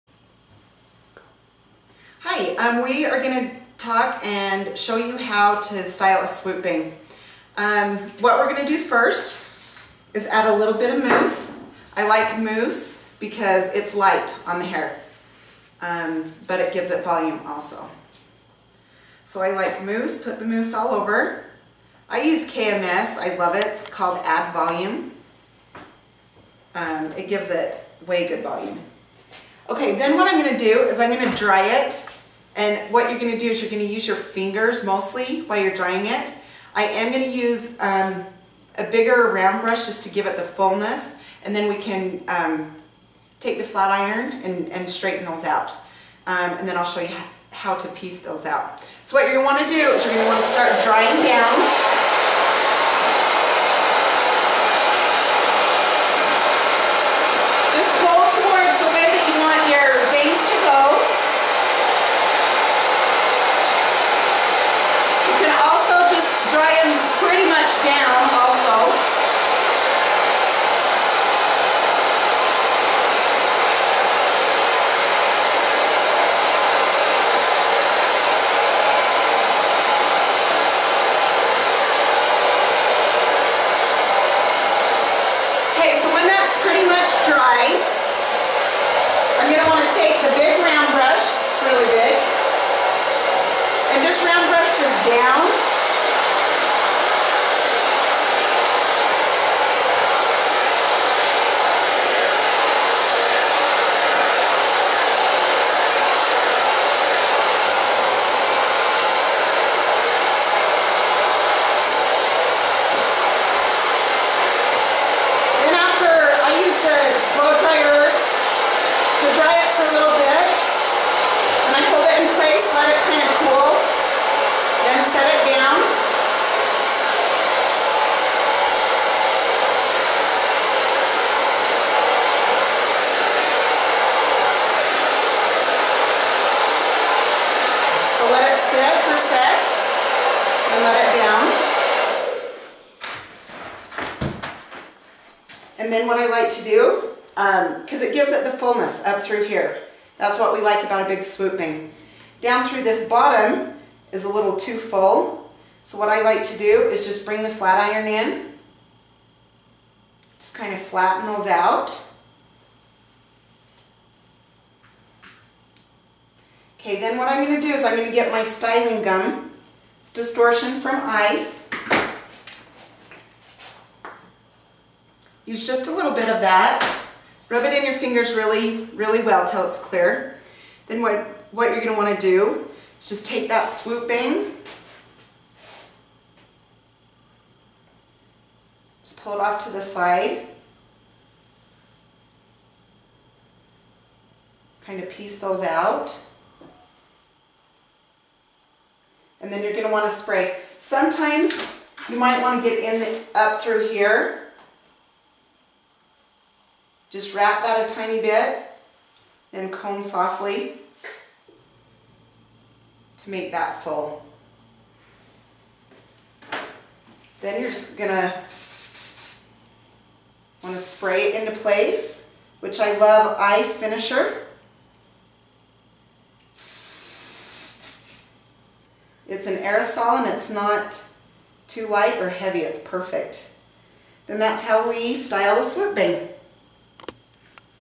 Side Swept Bangs Hair Tutorial By Radona
---Watch RaDona teach how To Cut Side Swept Bangs and Cut Side Bangs.

In this video, RaDona, a professional hairstylist shows you how to style a thick swoop bang.  How to Cute Hairstyles and how to do girls hairstyles.. is ...what we do.